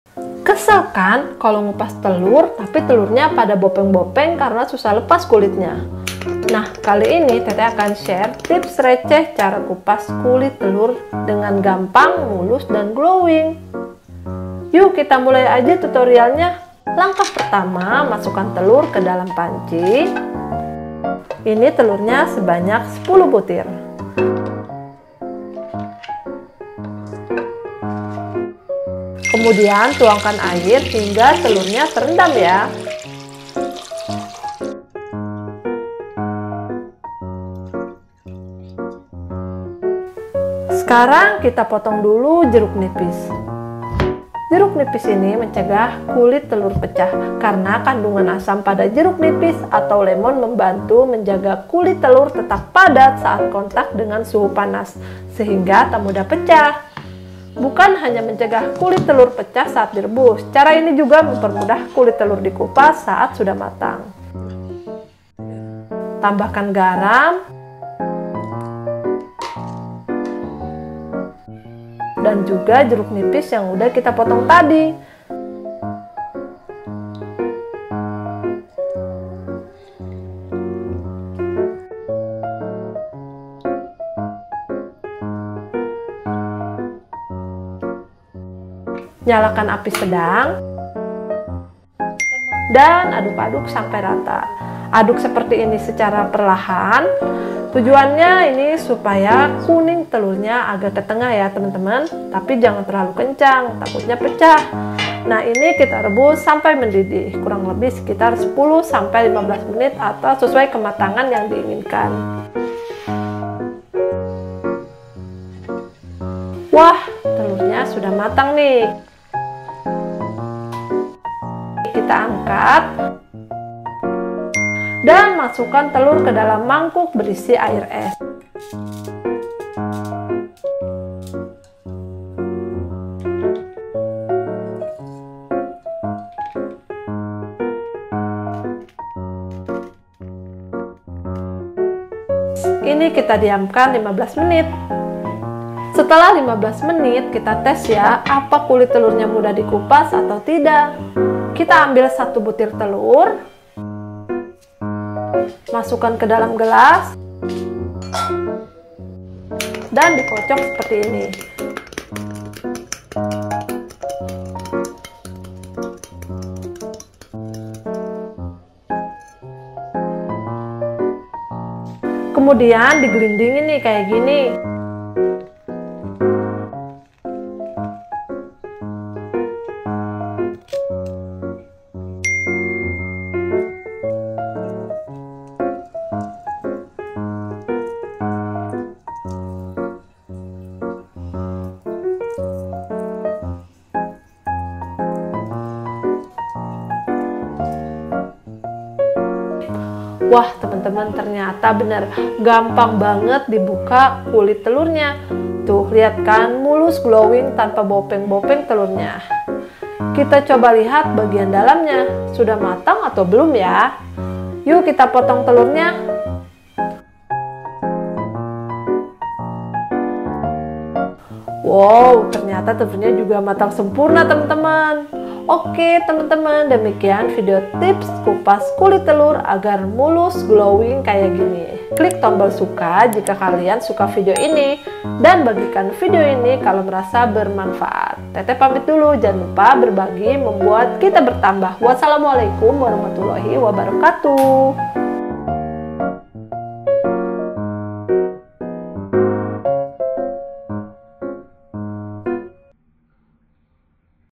TAMBAHIN 2 BUMBU INI SAAT REBUS TELUR , JADI GAMPANG DI KUPAS DAN GLOWING
Kesel kan kalo ngupas telur, tapi telurnya pada bopeng bopeng karena susah lepas kulitnya,
kali ini teteh akan share tips receh cara kupas kulit telur dengan gampang , mulus dan glowing.

Bahan yang di di siapkan :
- 10 butir Telur
- 2 sdt garam
- 1 buah jeruk nipis
- air secukupnya hingga telur terendam